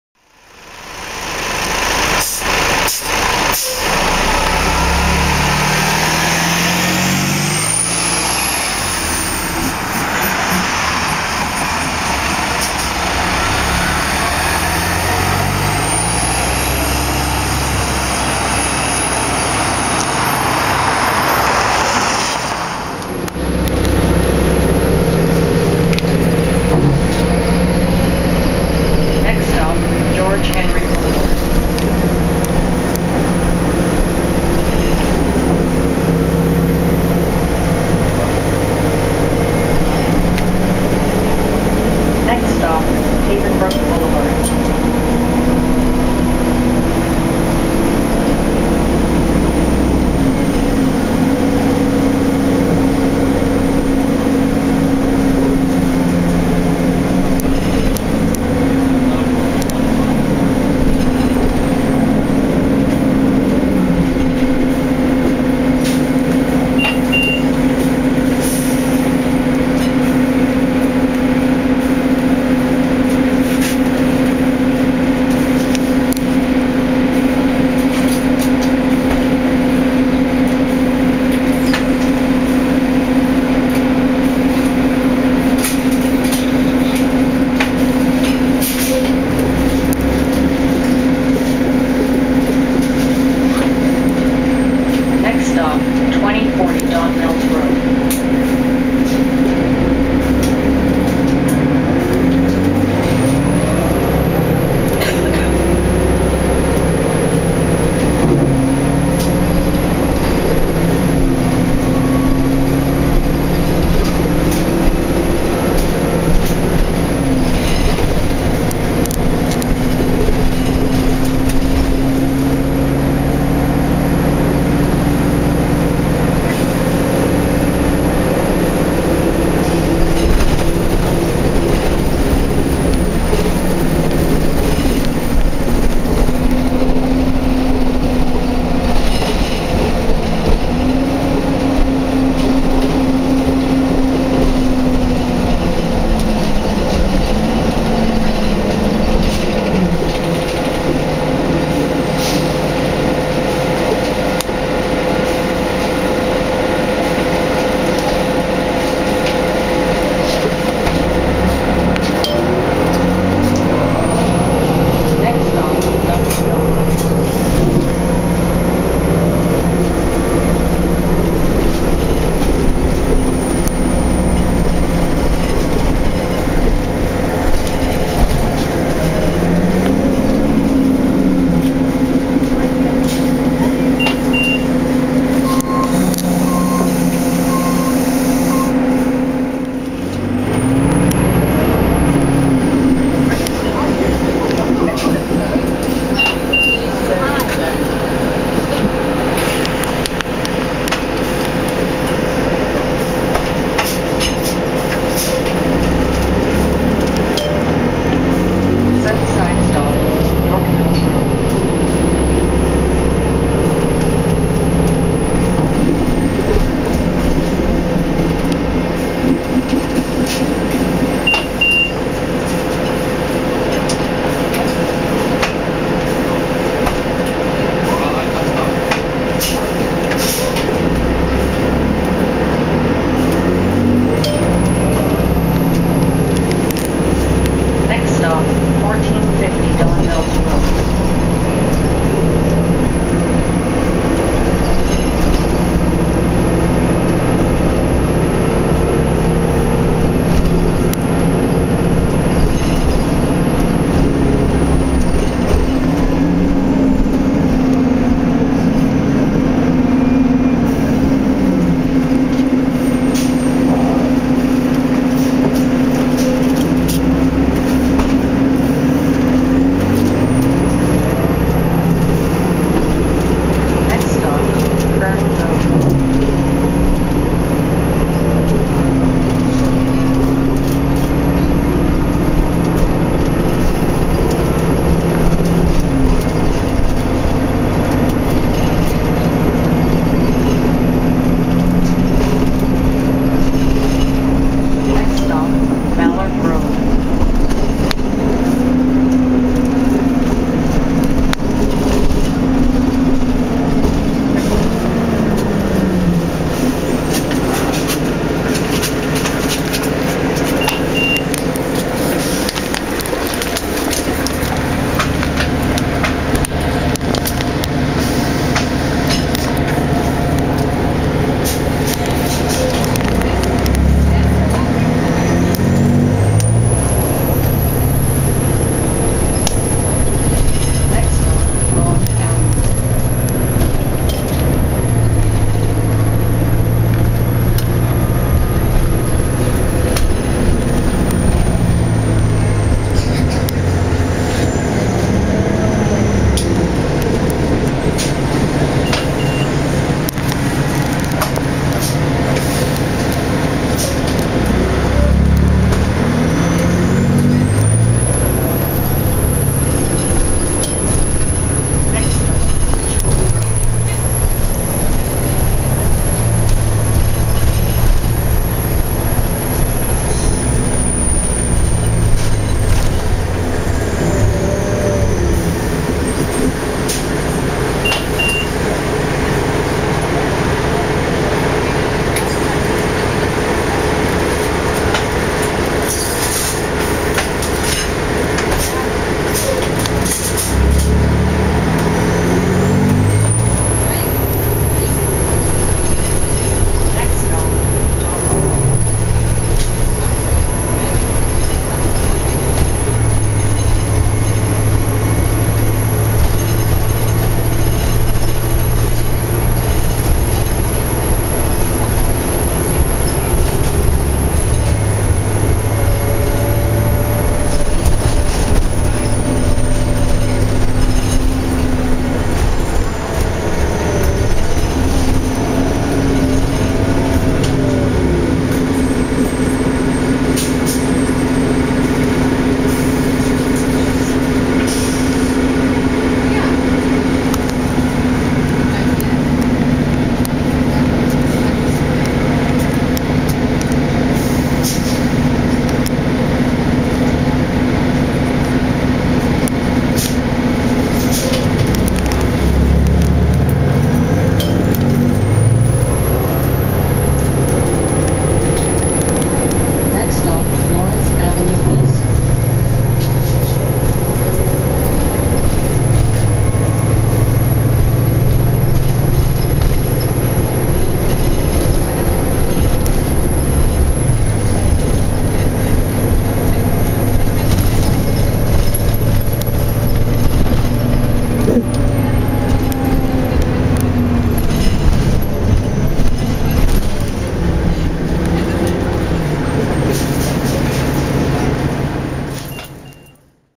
TTC - Ride Aboard 2003 Orion 07.501 VII Diesel #7490 on 25 Don Mills to Pape Subway Station
7490| 25 Don Mills to Pape Subway Station

Information about this vehicle:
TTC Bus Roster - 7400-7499
Year - 2003
Manufacturer - Orion Bus Industries-Daimler Buses North America
Model - Orion 07.501
Engine - Detroit Diesel Series 50EGR
Transmission - Allison B400R
Destination Sign - Luminator Horizon
Status - Retired